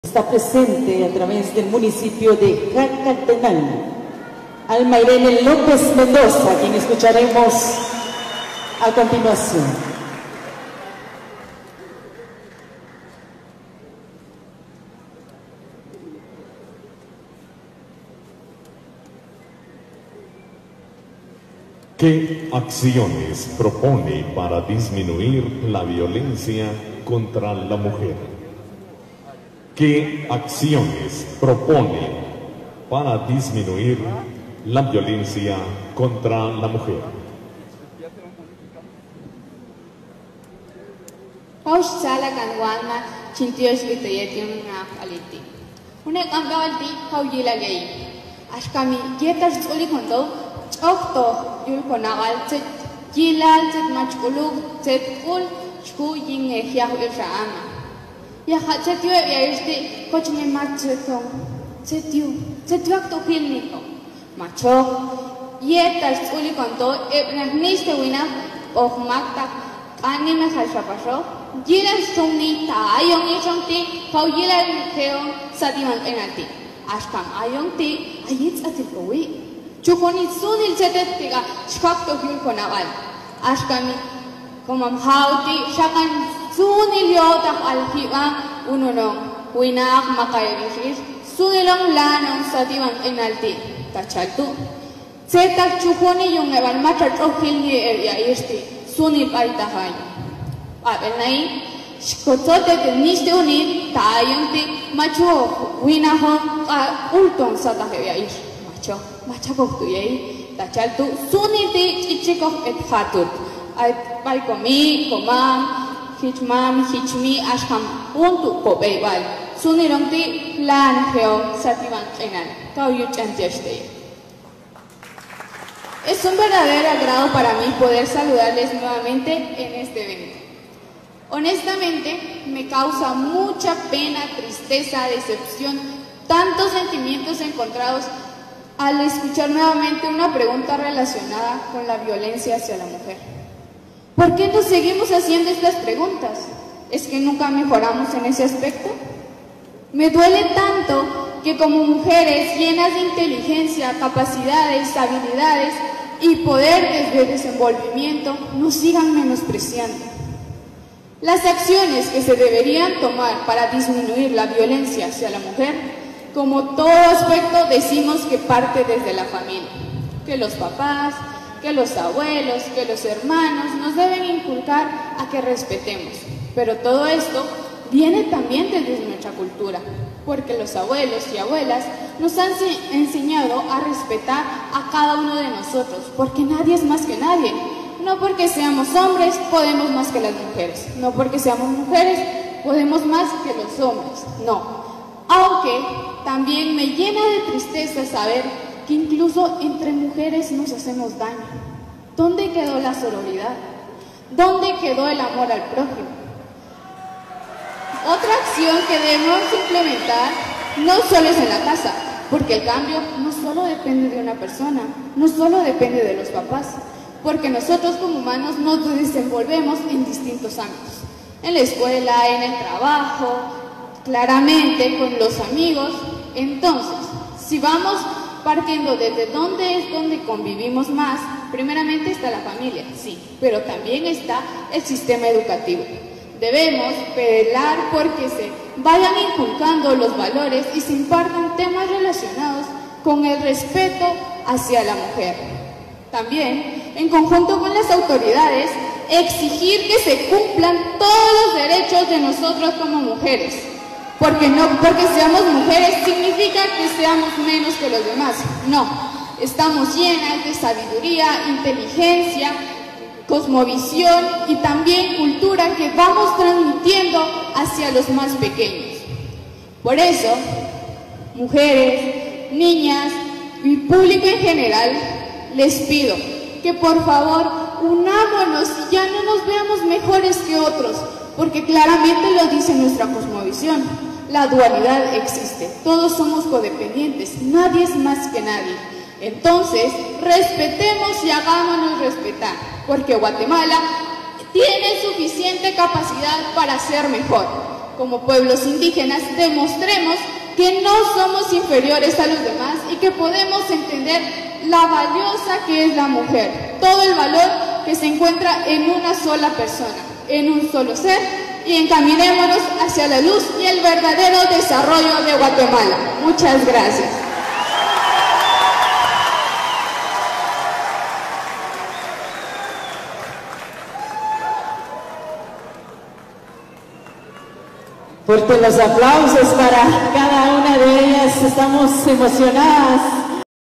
Emotivo mensaje de Alma Irene López Mendoza previo a ser coronada como la nueva Rab'in Ajaw 2022
Emotivo mensaje de Alma Irene López Mendoza previo a ser coronada como la nueva Rab'in Ajaw, número 51, septima corona para Huehuetenango y segunda para Jacaltenango desde aquel ya lejano 1985